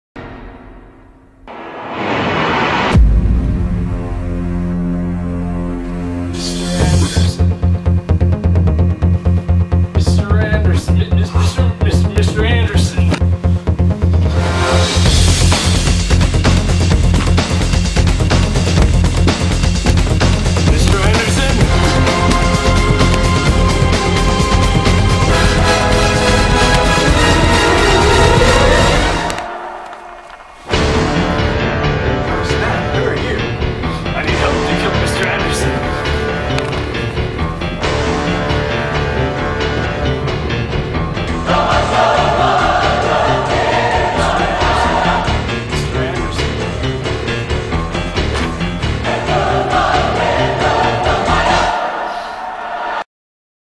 Matrix Parody
A really short film we made a while back testing our green screen.